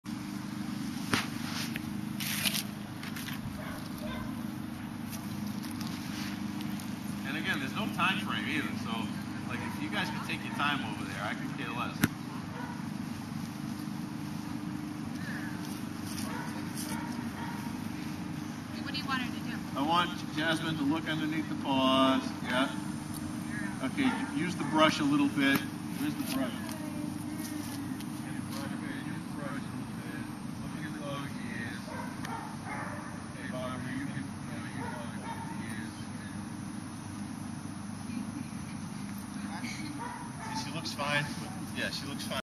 Caucasian Legend Anchara
Chara performing various parts of her CGC test taken in Nov 2007 @ 2.5 yr